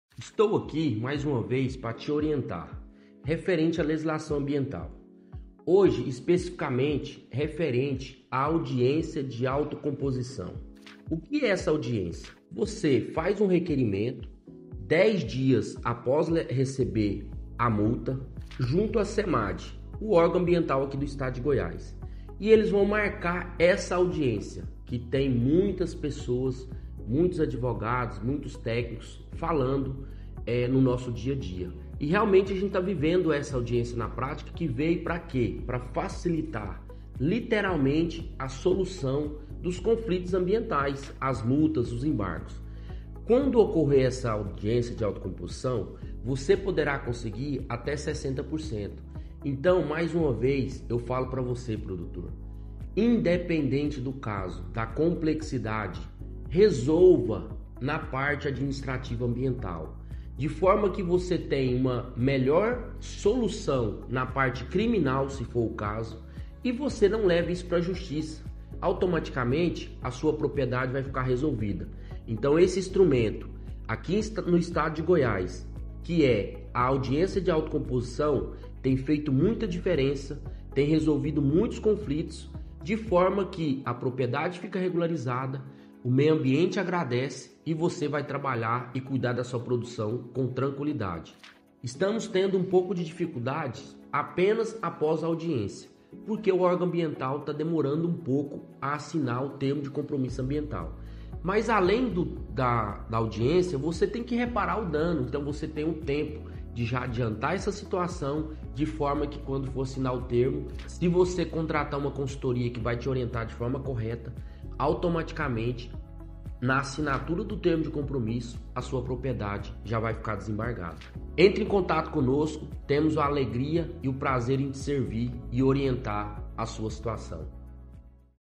Seu caso é grave???
Seu caso é grave??? Não importa tem solução

Pode ser resolvido em uma Audiência de AUTOCOMPOSIÇÃO

 Resolvendo os conflitos ambientais “Multas e Embargos”

Com base na Tríplice Responsabilidade “Civil, Criminal e Administrativa”, resolvendo na área administrativa, você terá sucesso rápido na parte Civil e Criminal

Empreendimento regularizado, você produz em paz, o meio ambiente agradece, nosso país, Brasil 🇧🇷, cresce, o mundo bate palmas 👏🏻

Esta ocorrendo apenas uma demora dificuldade somente após a ata de audiência, espera muito longa para assinatura do TCACM “Termo de Compromisso”, mas não fica depende do mais de você

E neste período pode correr agilizar os licenciamentos ambientais e reparação do dano, se for o caso